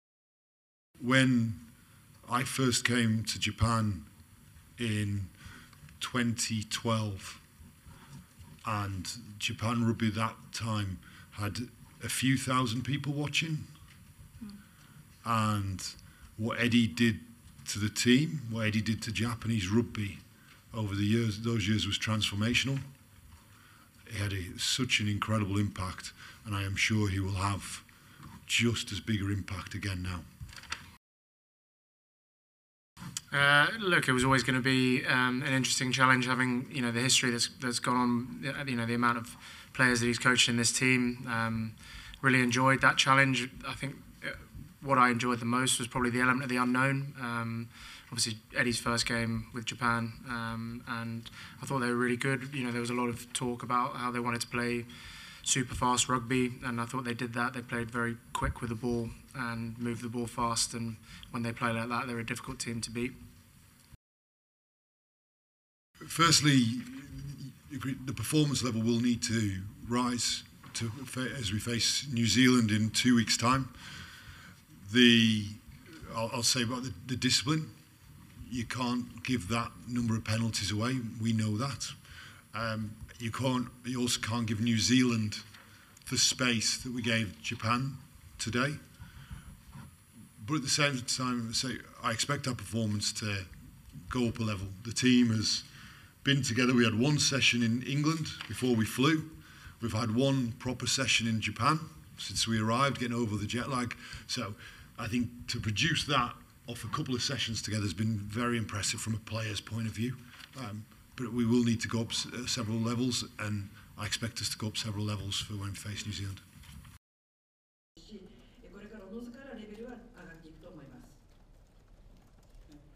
England coach Borthwick & captain George after beating Japan 52-17 in test match｜Rugby
Reactions from head coaches and captains after England beat Japan 52-17 in their test match in front of more than 40,000 spectators on Saturday at Japan National stadium in Tokyo.

It was Eddie Jones’ first test match after he returned as head coach of Japan.